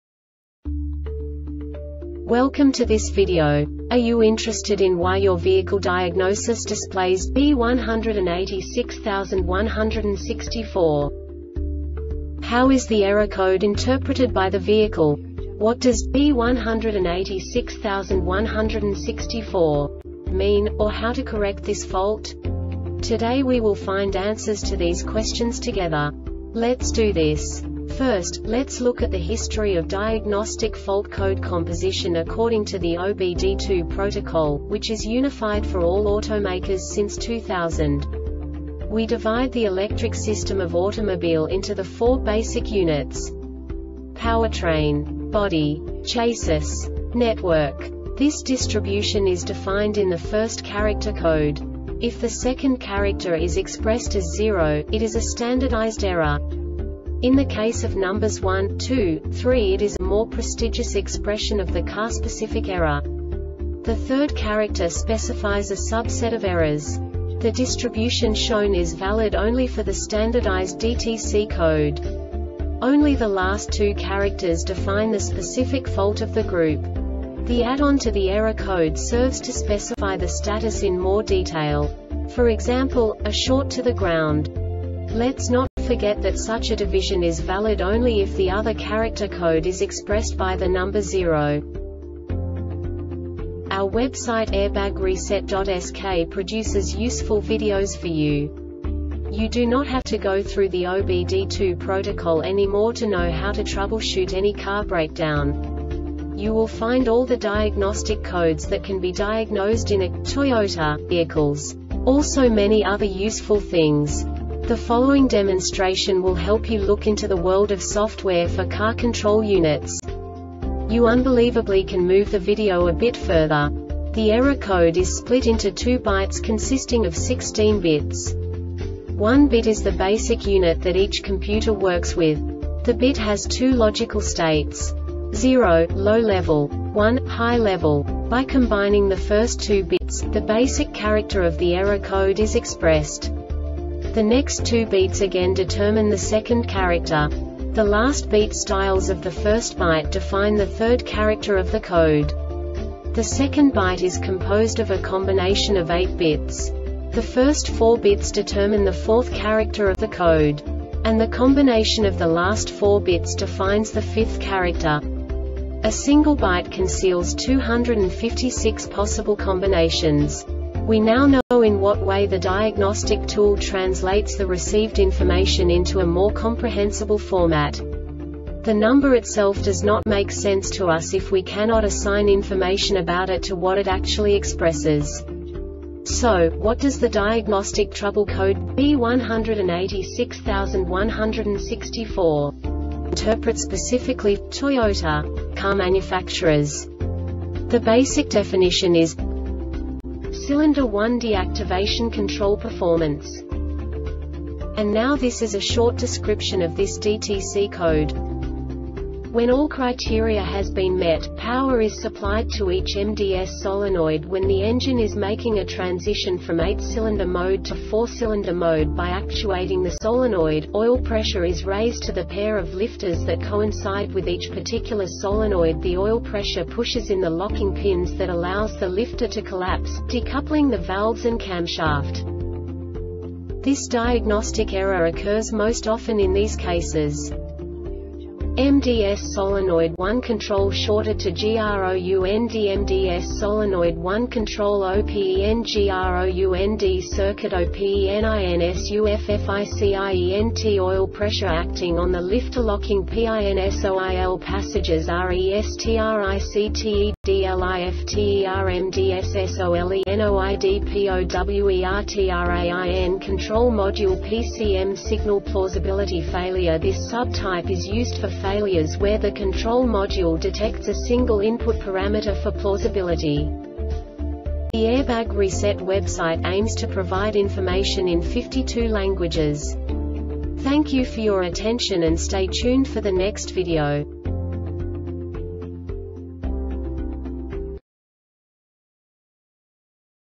DTC Toyota B1861-64 Short Explanation
Contents:
0:21 Basic DTC analysis according to OBD2 protocol standard.
1:48 Insight into programming
2:58 A diagnostically understandable description of the Diagnostic Trouble Code
3:05 Basic error definition

Make:
Toyota
Code:
B1861 64
Definition:
Cylinder 1 Deactivation Control Performance
Description:
When all criteria has been met, power is supplied to each MDS Solenoid when the engine is making a transition from 8 cylinder mode to 4 cylinder mode. By actuating the solenoid, oil pressure is raised to the pair of lifters that coincide with each particular solenoid. The oil pressure pushes in the locking pins that allows the lifter to collapse, decoupling the valves and camshaft.
Cause:
MDS SOLENOID 1 CONTROL SHORTED TO GROUNDMDS SOLENOID 1 CONTROL OPENGROUND CIRCUIT OPENINSUFFICIENT OIL PRESSURE ACTING ON THE LIFTER LOCKING PINSOIL PASSAGES RESTRICTEDLIFTERMDS SOLENOIDPOWERTRAIN CONTROL MODULE (PCM)
Failure Type: